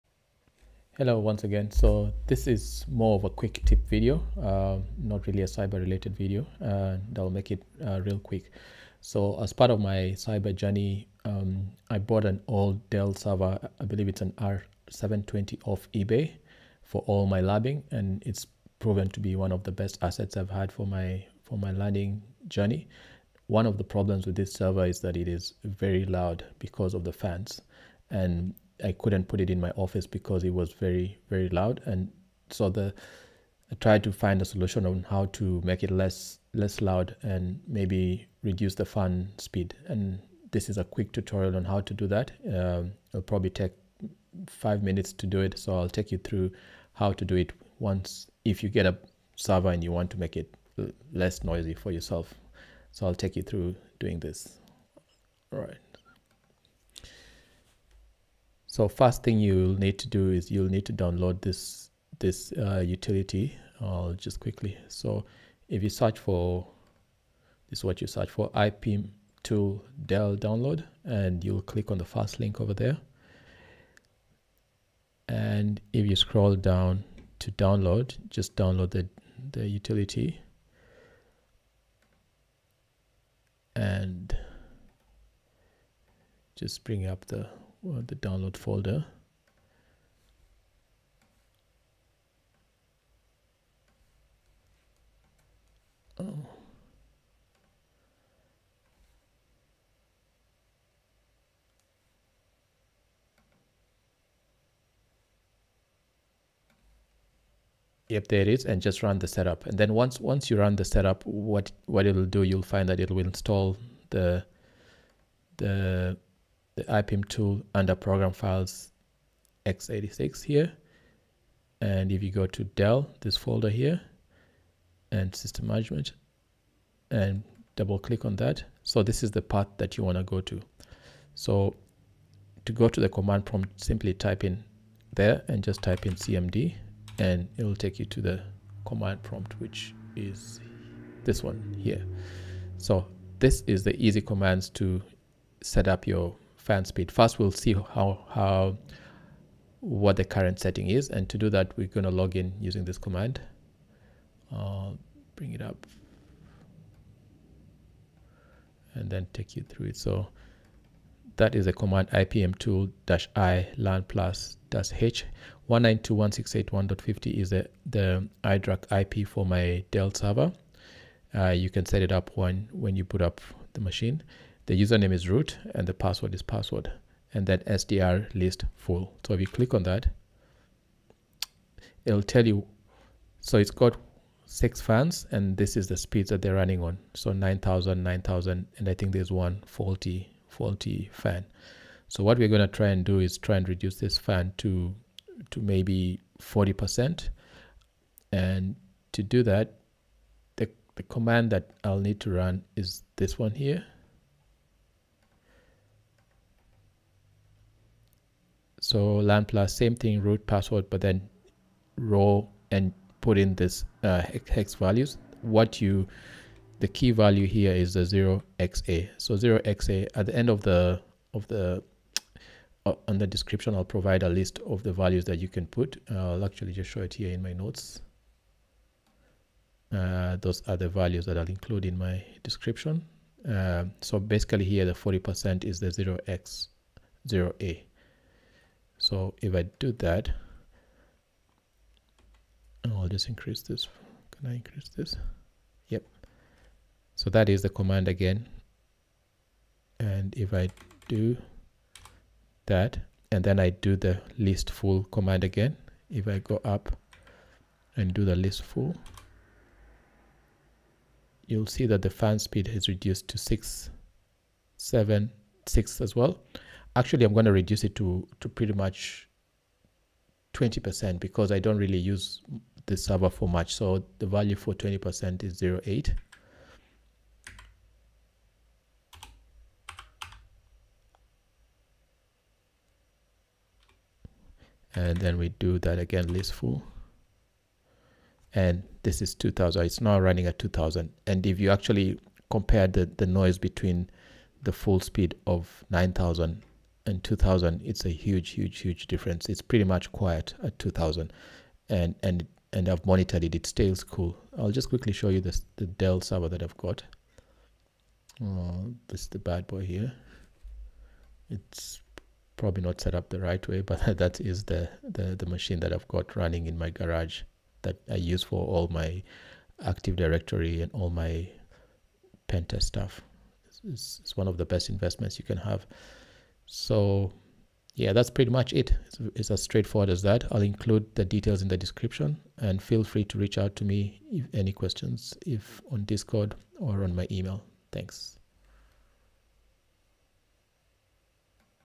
How to Set Fan Speeds on Dell Servers | Reduce Noise Easily
Is your Dell server making too much noise? Learn how to take control of your server's fan speeds using ipmitool. In this tutorial, I’ll show you how to adjust fan speeds to specific percentages, optimize cooling, and dramatically reduce noise levels in your server setup.

By controlling fan speeds manually, you can:

- Quiet down noisy server fans.
- Balance cooling efficiency with sound levels.
- Maintain optimal server performance without unnecessary noise.

# Set Fan Speed to 10%
 raw 0x30 0x30 0x02 0xFF 0x04

# Set Fan Speed to 20%
 raw 0x30 0x30 0x02 0xFF 0x08

# Set Fan Speed to 30%
 raw 0x30 0x30 0x02 0xFF 0x0C

# Set Fan Speed to 40%
 raw 0x30 0x30 0x02 0xFF 0x0A

# Set Fan Speed to 50%
 raw 0x30 0x30 0x02 0xFF 0x10

# Set Fan Speed to 60%
 raw 0x30 0x30 0x02 0xFF 0x14

# Set Fan Speed to 70%
 raw 0x30 0x30 0x02 0xFF 0x18

# Set Fan Speed to 80%
raw 0x30 0x30 0x02 0xFF 0x1C

# Set Fan Speed to 90%
 raw 0x30 0x30 0x02 0xFF 0x20

# Set Fan Speed to 100%
raw 0x30 0x30 0x02 0xFF 0xFF